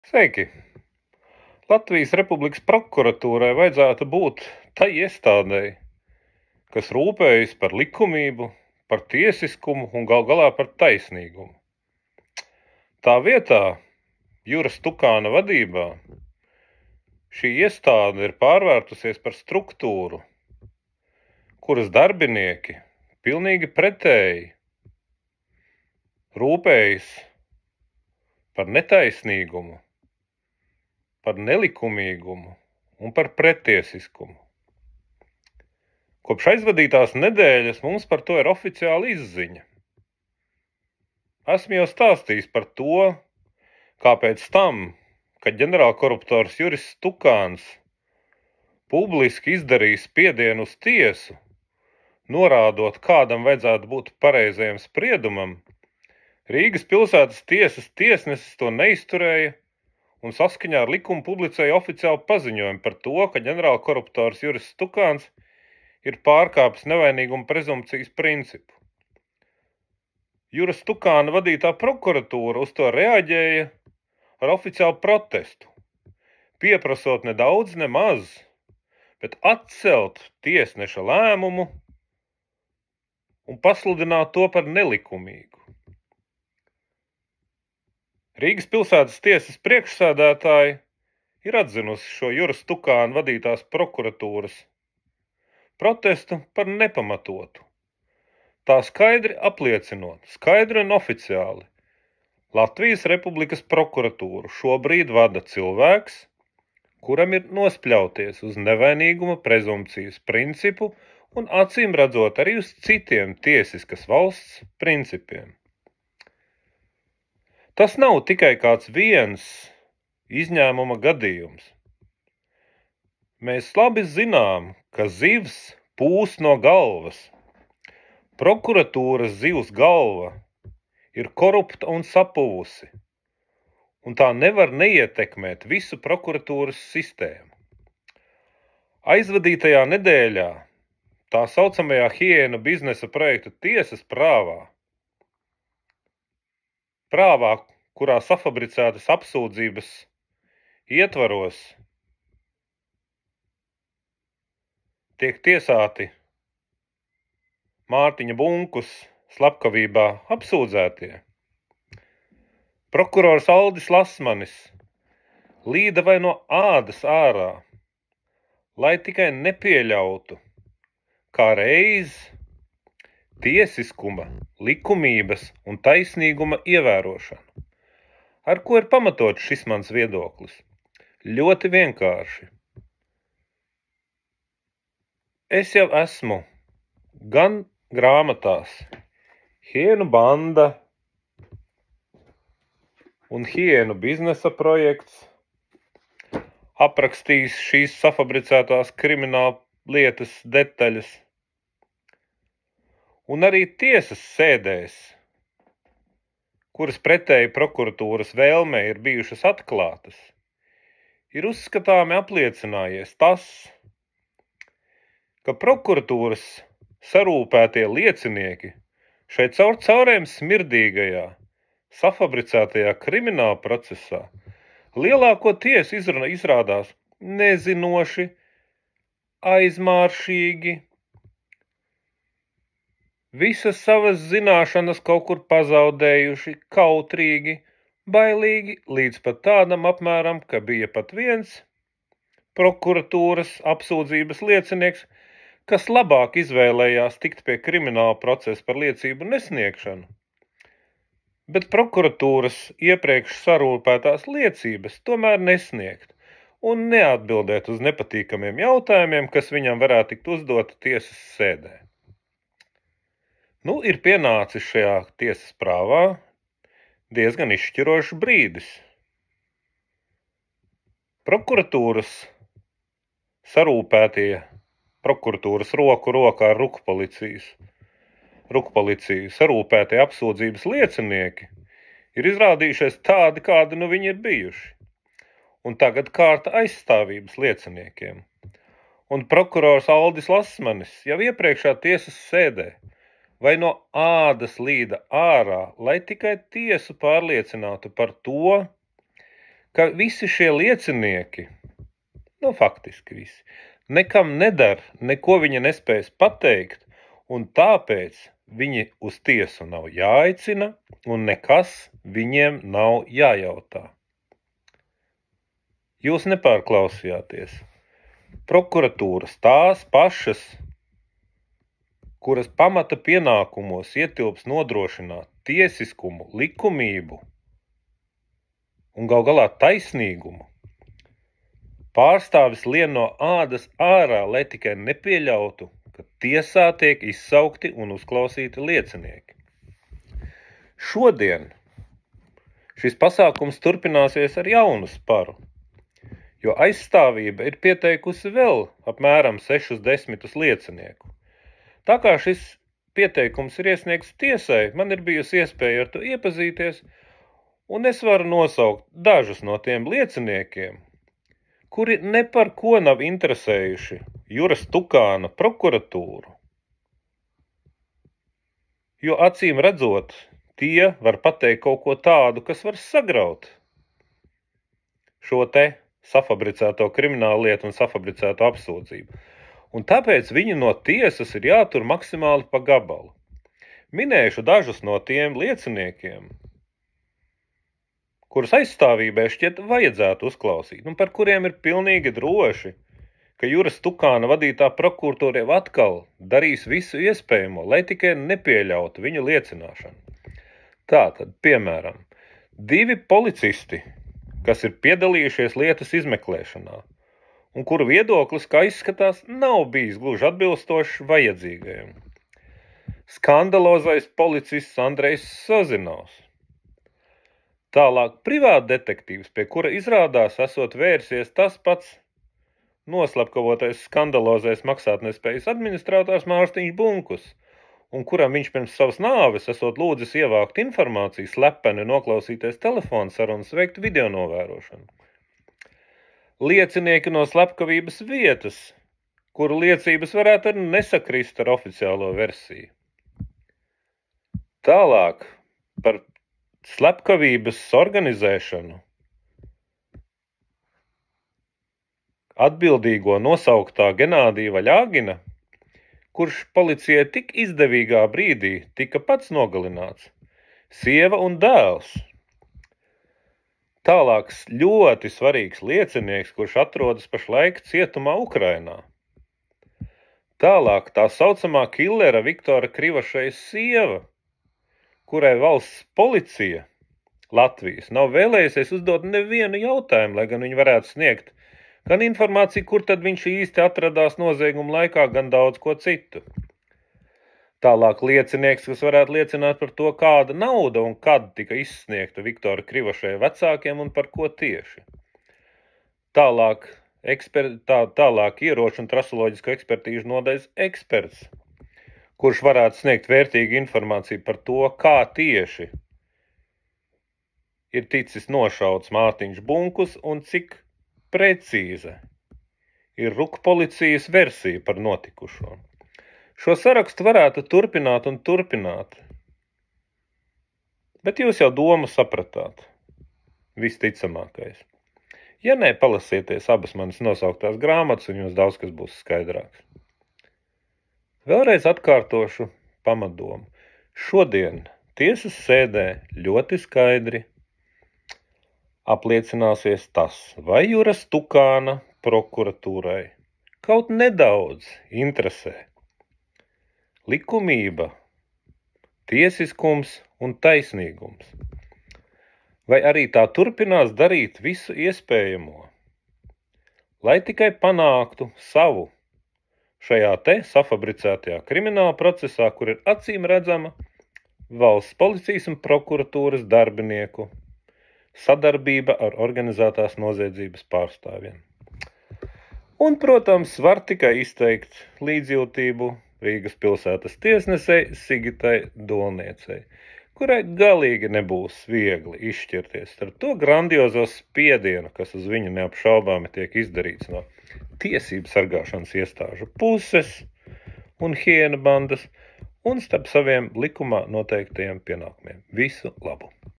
Kāpēc Stukāna prokuratūra lien vai no ādas ārā, lai nepieļautu “nepareizu” liecinieku uzklausīšanu
Hiēnu biznesa projekta prāva: kāpēc Stukāna prokuratūra lien vai no ādas ārā, lai nepieļautu “nepareizu” liecinieku uzklausīšanu